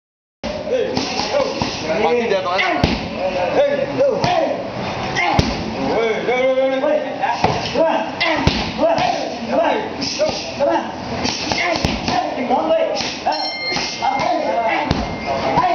Dom Kicking The Bag
Dom kicking the bag whilst Kru Sit watches. Dom had a fight soon after this and won hands down. Filmed at sasiprapa April 2006